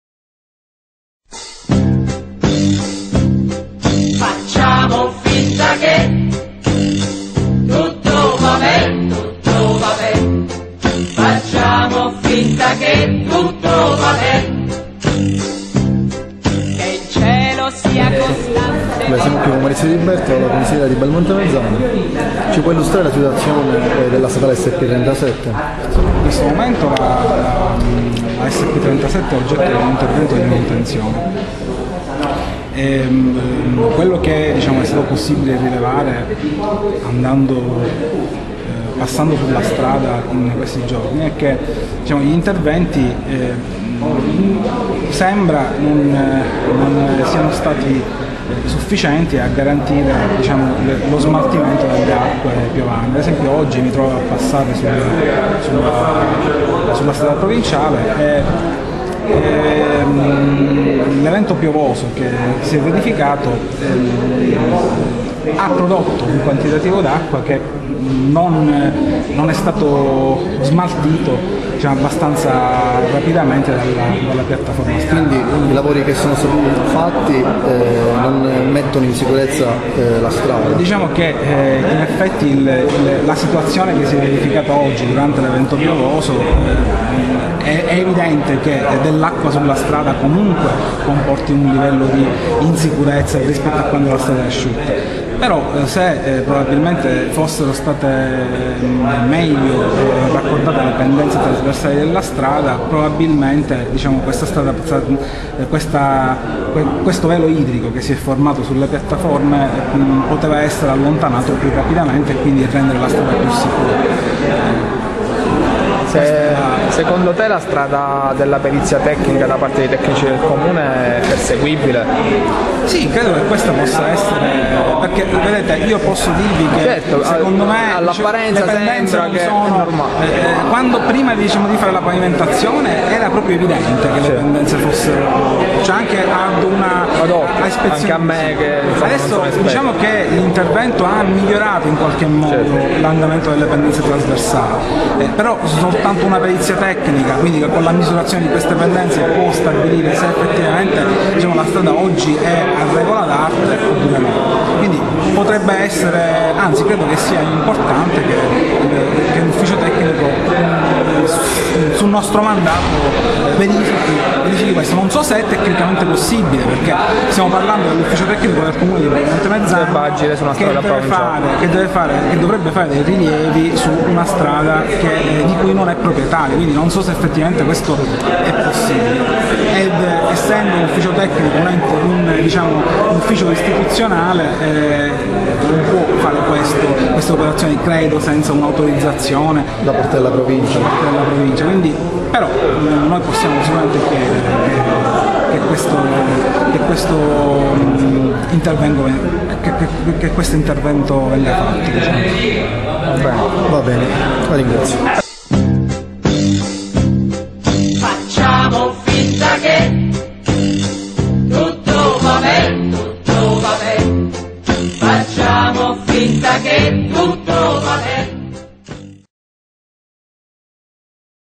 Intervista a Maurizio Di Liberto - Sicurezza Stradale SP37
In occasione del consiglio comunale di Belmonte Mezzagno abbiamo intervistato il consigliere Maurizio Di Liberto in merito alla situazione della strada provinciale 37 che collega il paese a Palermo. Troppi incidenti e poca chiarezza.